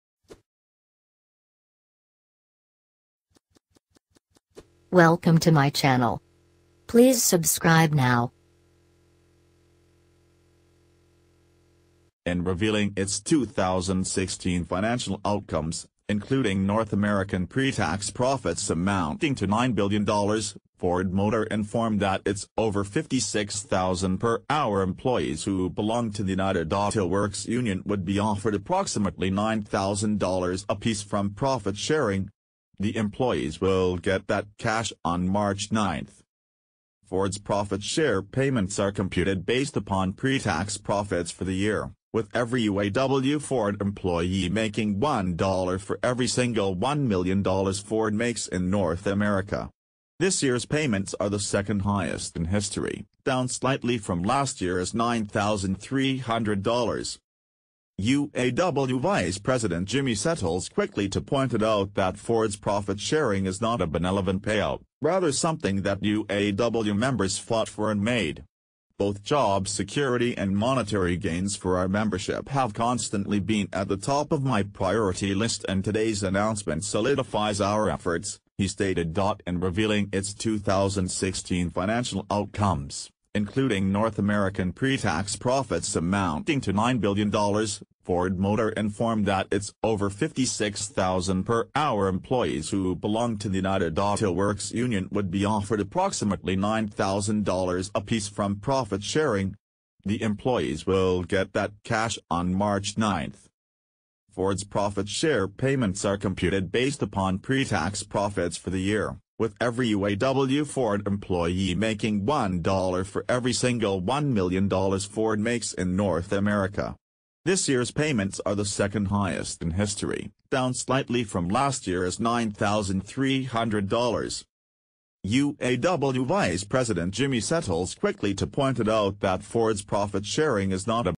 Ford To Provide 56,000 Hourly Workers $9,000 From Profit Sharing
In revealing its 2016 financial outcomes, including North American pre-tax profits amounting to $9 billion, Ford Motor informed that its over 56,000 per hour employees who belong to the United Auto Works union would be offered approximately $9,000 apiece from profit sharing. The employees will get that cash on March 9th.

Ford’s profit share payments are computed based upon pre-tax profits for the year, with every UAW-Ford employee making $1 for every single $1 million Ford makes in North America. This year’s payments are the second-highest in history, down slightly from last year’s $9,300.

UAW Vice President Jimmy Settles quickly to pointed out that Ford’s profit sharing is not a “benevolent payout,” rather something that UAW members fought for and made. “Both job security and monetary gains for our membership have constantly been at the top of my priority list and today’s announcement solidifies our efforts,” he stated.

0